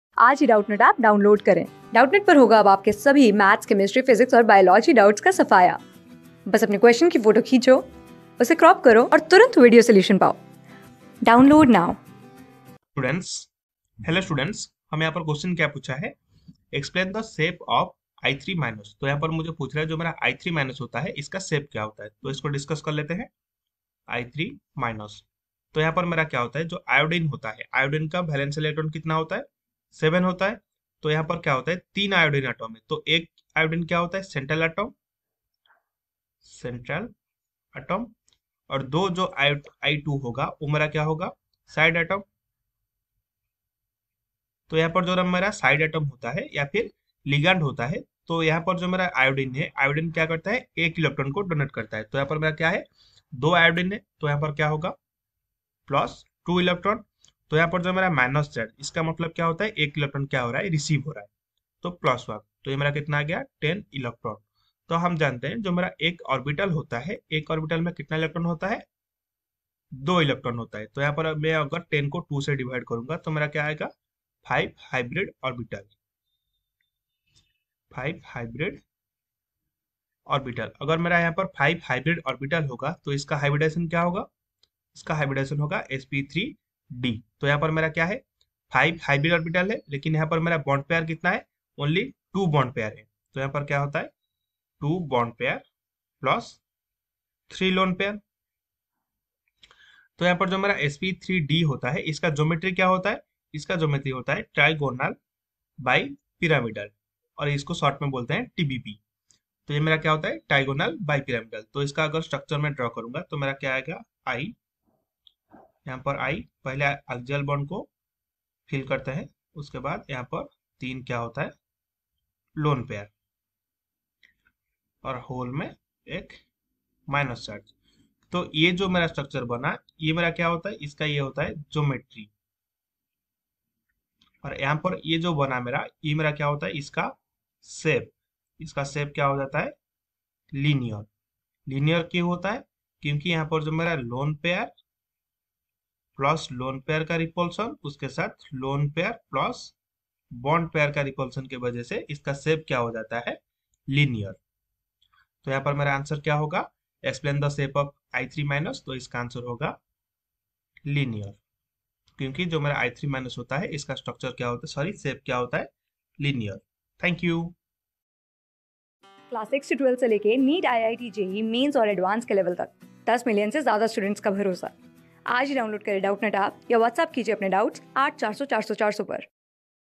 Explain the shape of I_(3)^(-) ion . | CLASS 11 | CHEMICAL BONDING AND MOLECULAR STRUCTURE | CH...
Explain the shape of I_(3)^(-) ion .
Class: 11
Subject: CHEMISTRY
Chapter: CHEMICAL BONDING AND MOLECULAR STRUCTURE
Board:CBSE

👉 Have Any Query? Ask Us.